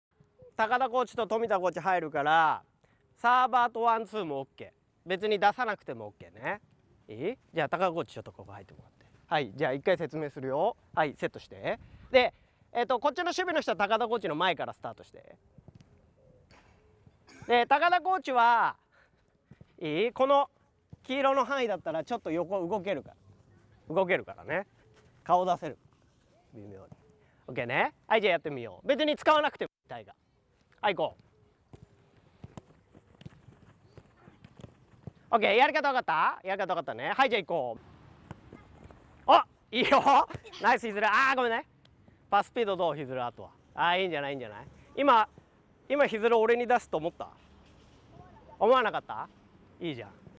川崎フロンターレU-10の「運ぶドリブル」を身に付けるトレーニング4
「１対４＋１サーバー」

「１対４」のシチュエーションに、サーバーが加わる。サーバーへのパスを匂わせることで、守備の選手をかく乱することができる。

▼この動画の全編はCOACH UNITED ACADEMYで配信中！詳しくはこちら

【講師】冨田 幸嗣／
川崎フロンターレU-10コーチ。選手時代は川崎フロンターレU-15、U-18でプレー。その後、川崎フロンターレのスクールコーチなどを経て、2014年からU-10のコーチを務めている。